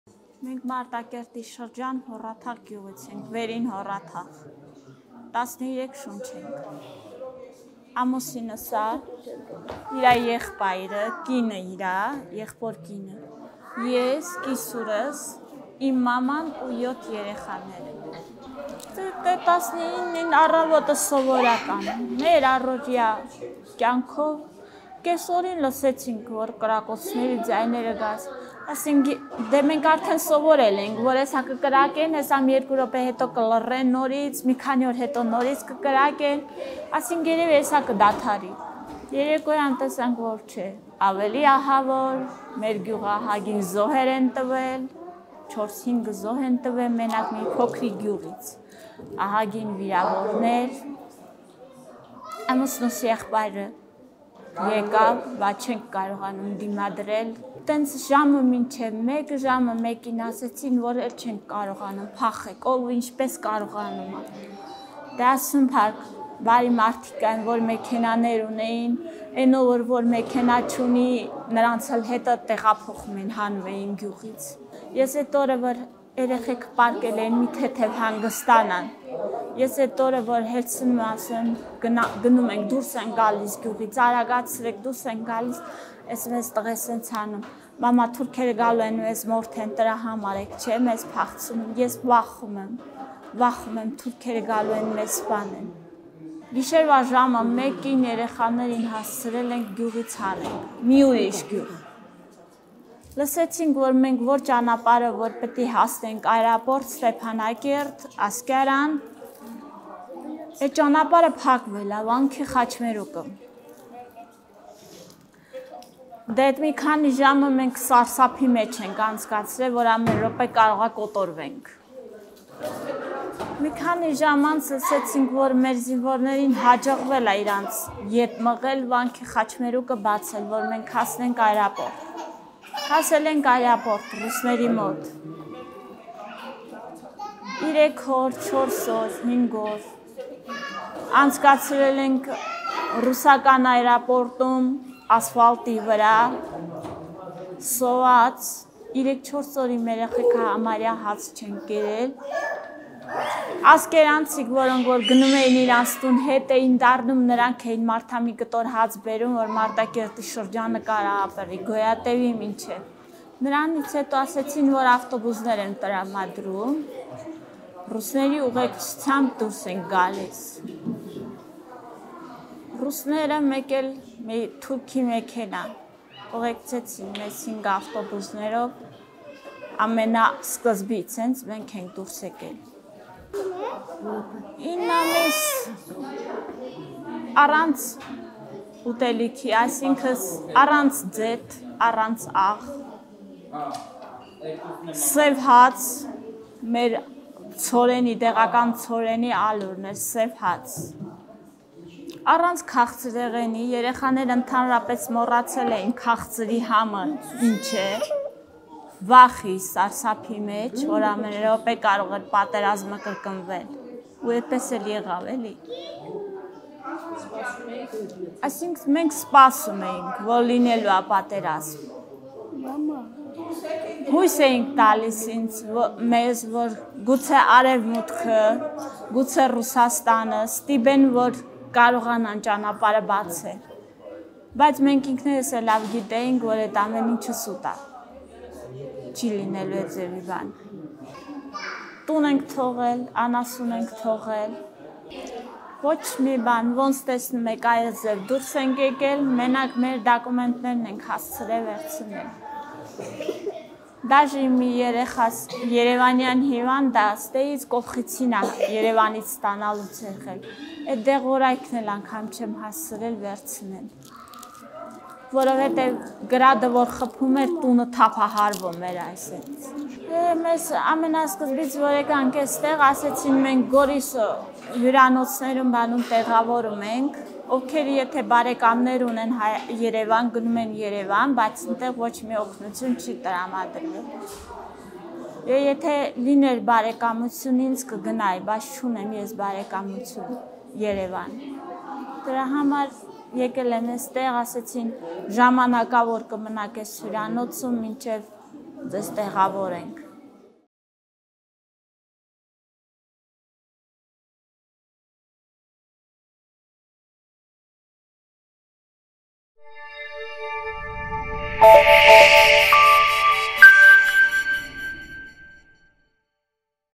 «Ով ոնց կարողացավ փախավ գյուղից»․ արցախցի 7 երեխաների մայր
28-ամյա Արփինե Հովհաննիսյանը ընտանիքը՝ ամուսինը, 7 երեխաները, ամուսնու եղբայրը, նրա կինը, սկեսուրը և մայրը տեղահանվել են Արցախի Մարտակերտի շրջանի Վերին Հոռաթաղ գյուղից։

«Մի քանի ժամ ճանապարհին կանգնել ենք։ Էդ մի քանի ժամը մենք սարսափի մեջ ենք անցկացրել, որ ամեն րոպե կարող է կոտորվենք։ Մի քանի ժամ անց լսեցինք, որ մեր զինվորներին հաջողվել է հետ մղել ադրբեջանցիներին և Վանքի խաչմերուկը բացել, էդպես կարողացանք հասնել օդանավակայան։ Այնտեղ ռուս խաղաղապահների բազայում 3-4 օր անցկացրինք՝ ասֆալտի վրա, սոված, 3-4 օր իմ երեխաները համարյա բան չեն կերել։ Դրանից հետո ավտոբուսներ տրամադրեցին, ռուսների, մեկ էլ մի ադրբեջանական մեքենայի ուղեկցությամբ դուրս ենք եկել։ Առաջիններից մեկն էինք, որ սեպտեմբերի 24–ին Արցախից եկանք Հայաստան»,-պատմում է Արփինեն։